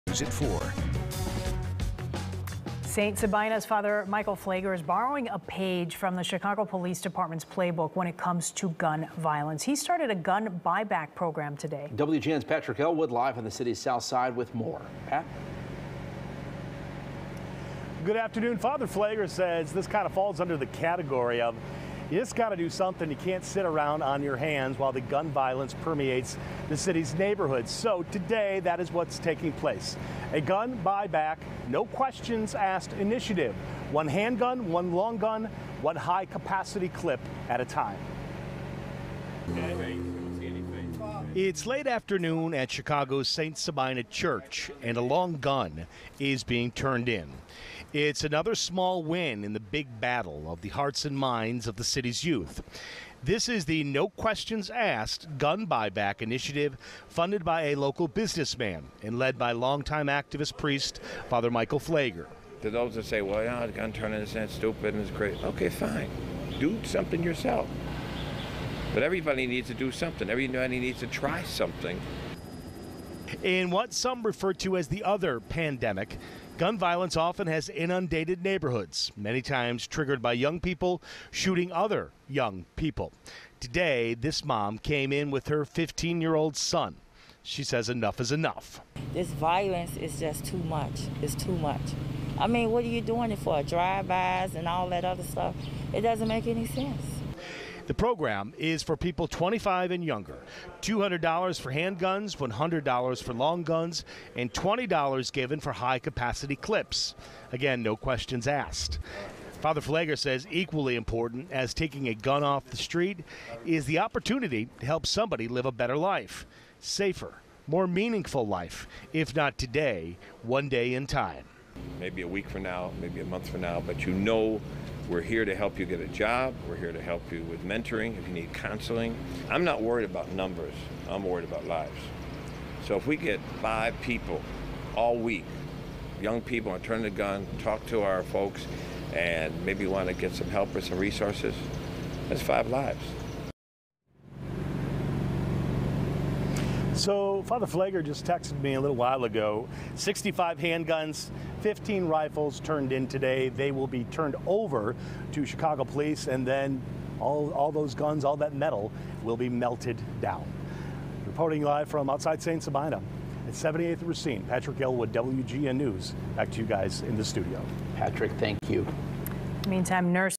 Gun buyback program kicks off Monday at St. Sabina Church
The buyback program will run Monday through Friday from 8 a.m. to 4 p.m.